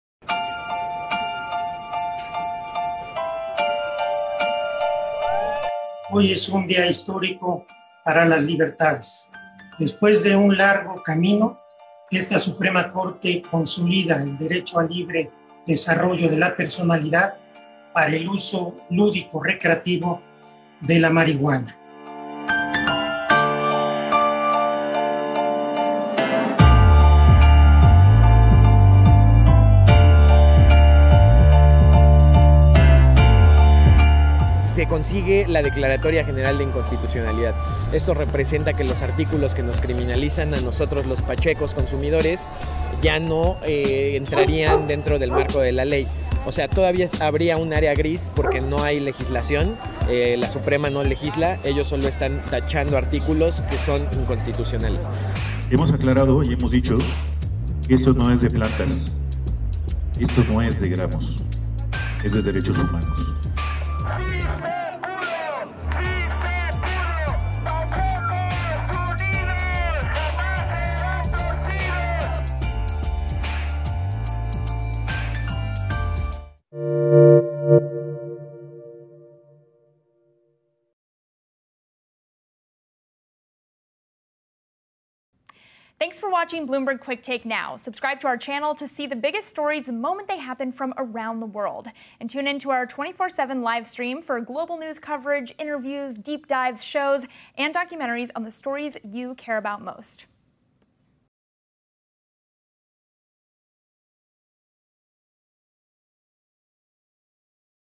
Mexico's Supreme Court Removes Ban Against Smoking Marijuana
Mexico's Supreme Court has ordered the government to start issuing permits to grow limited amounts of marijuana after the country's Congress took too long to approve a limited legalization law.

This does not mean that marijuana is legalized, but it will mean that any Mexican who asks for a permit will be able to consume it legally if they grow it at home and under certain restrictions.

The Court's resolution leaves it in the hands of the Ministry of Health and the Federal Commission for Protection against Health Risks to establish the details of the permits for self-consumption.

In 2019, the Court ruled that prohibiting marijuana was unconstitutional and gave lawmakers until April 30 to pass a law.

In March, the lower house approved a marijuana legalization bill, but it bogged down in the Senate.

The ruling was considered a clear wake-up call to the Mexican Congress, which has been unable to legislate on the issue despite the high Court's demands on various occasions since 2019.

"Today is a historic day for freedoms," said the President of the Supreme Court, Arturo Zaldívar. "After a long road, this Supreme Court recognizes the right to the free development of personality for the recreational use of marijuana".

Next to the Supreme Court headquarters, members of the Mexican Cannabis Movement celebrated the Court's decision and hoped that the criminalization of those who smoke marijuana will end from now on.